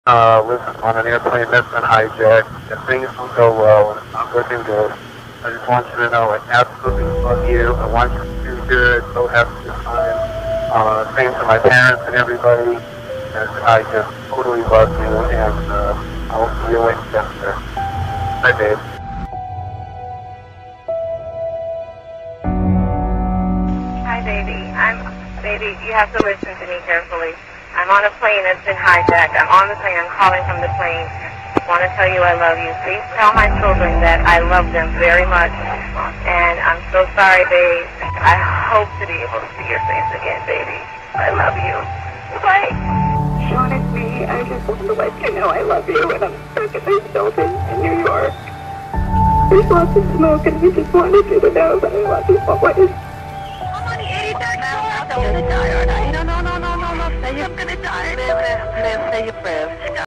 #🔴🔞🏴🎥The sounds you hear are real just before the plane hit the building on 9/11...
It has been 20 years since the terrorist attacks of Al Qaeda on September 11 in America.

 👈 19 members of the al-Qaeda fundamentalist group hijacked four passenger planes and crashed two planes into the twin towers of the World Trade Center in New York, ‌ one hit the US Department of Defense building and one was shot down.

 As a result, 2,993 people (along with the occupants of the planes) were killed and about 25,000 were injured, both of which destroyed the World Trade Center two hours later.  The United States invaded Afghanistan to attack al-Qaeda and ousted the same Taliban with whom it has now agreed to seize power.

 ک